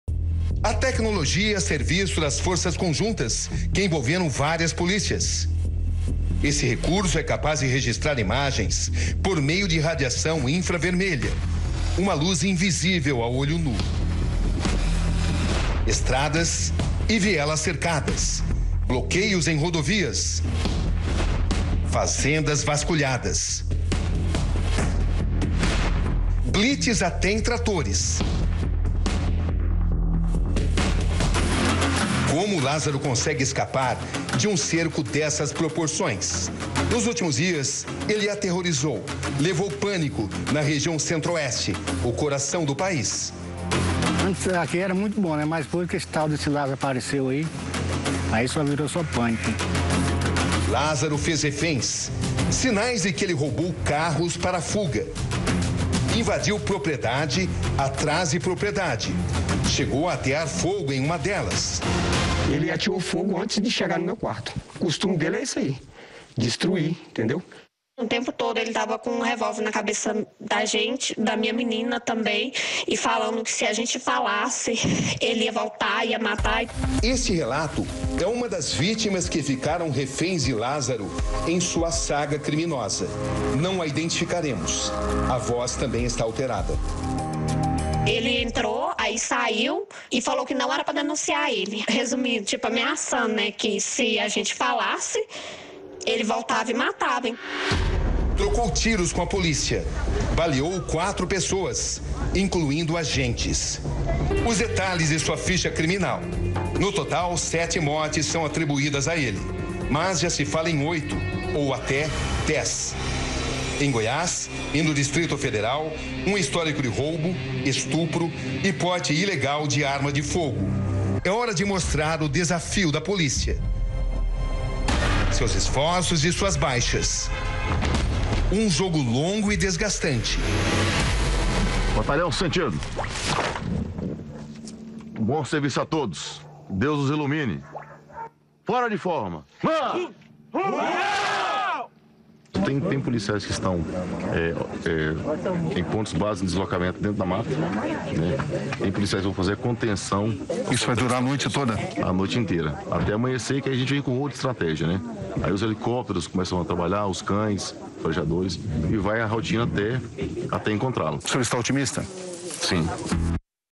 CAÇADA À LÁZARO: POLÍCIA COMPLETA TREZE DIAS DE TRABALHO NA MATA
O REPÓRTER ROBERTO CABRINI TAMBÉM ACOMPANHOU O TRABALHO DOS POLICIAIS NA REGIÃO DE COCALZINHO DE GOIÁS EM BUSCA DE LÁZARO BARBOSA.